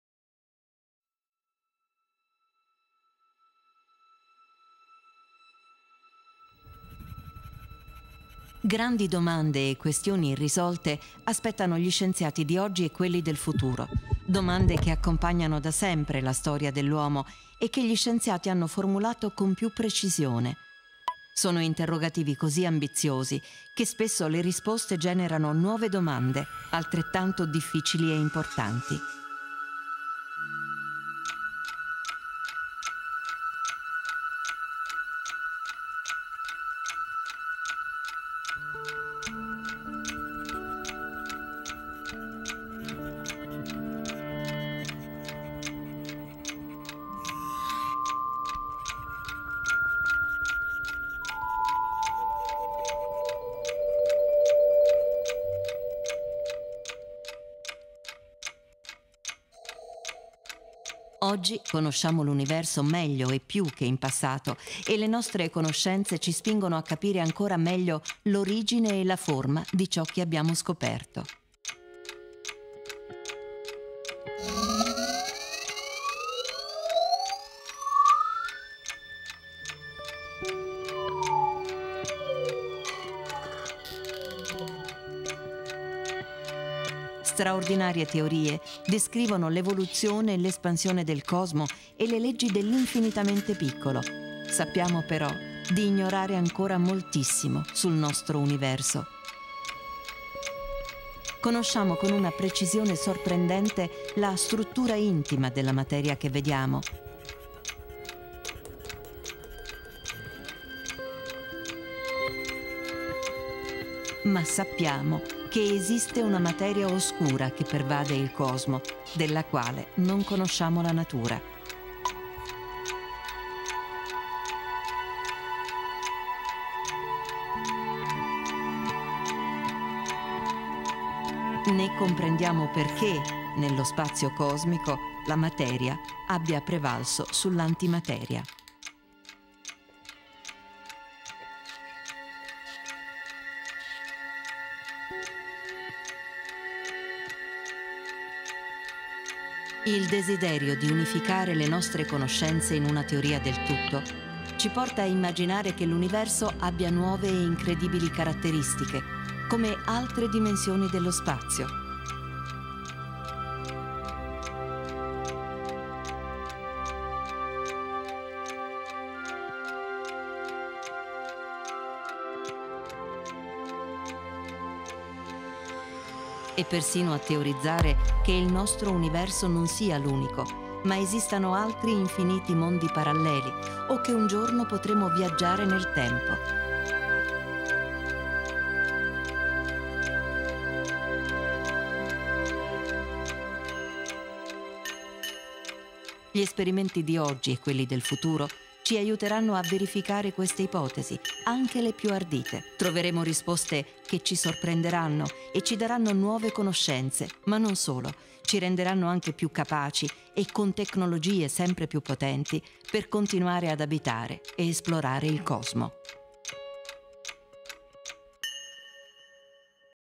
Sfide e domande ancora aperte sull' universo
Astri e Particelle: Sono molte le domande sulla struttura e l'origine dell' Universo che aspettano una risposta. La fisica e l'astrofisicia, studiando l'infinitamete piccolo, le particelle, e l'infinitamente grande, il cosmo, cercano le risposte a queste domande.